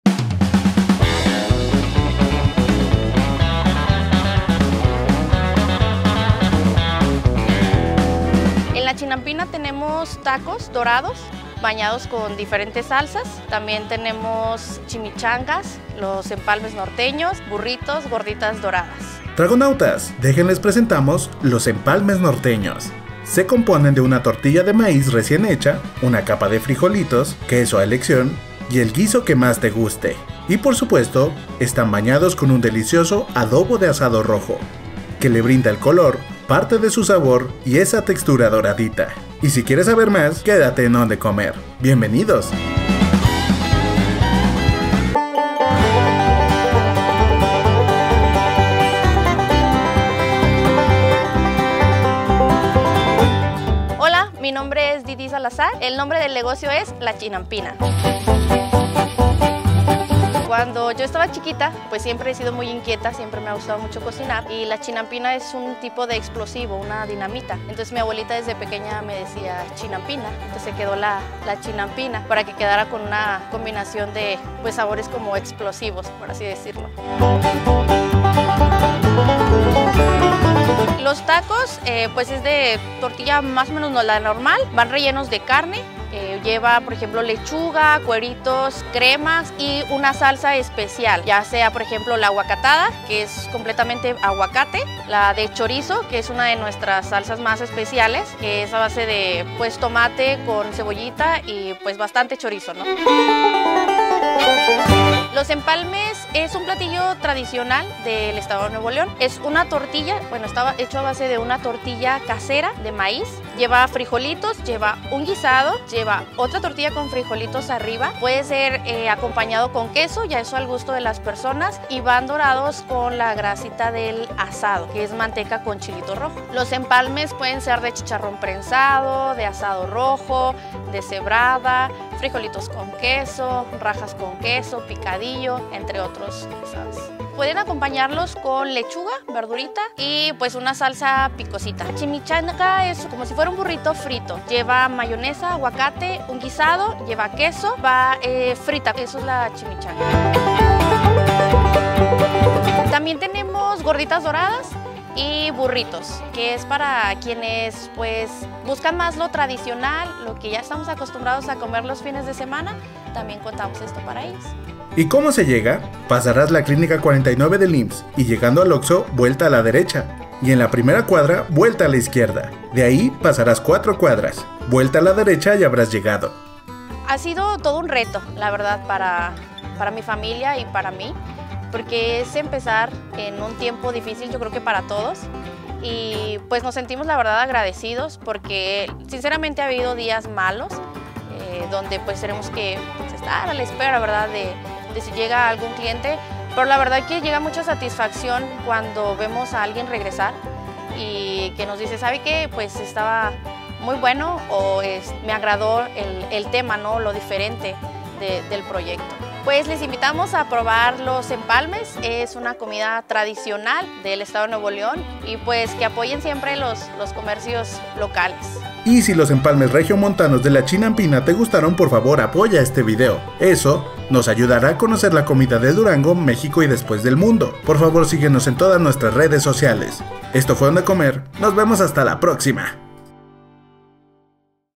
Los Empalmes de la Chinampina | Onde Comer Durango
En el episodio de hoy, los Tragonautas conocerán los empalmes, un platillo del estado de Nuevo León, ademas podrán probar taquitos dorados con una imperdible salsa de chorizo. Todo esto lo encontrarás en la Chinampina.

CONOCE MÁS:
🏠 Dirección: Calle Pelícano 103 Fracc. Real del Mezquital
⏰ Horarios: Jueves a Sábado 7:00 pm a 11:00 pm
🛵 Servicio a Domicilio: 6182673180 DiDiFood
💳 Pagos: Efectivo• Tarjeta • Transferencias
🥙 Opción Vegetariana: No
🍺 Bebidas Alcohólicas: No
🌭 Menú Infantil: No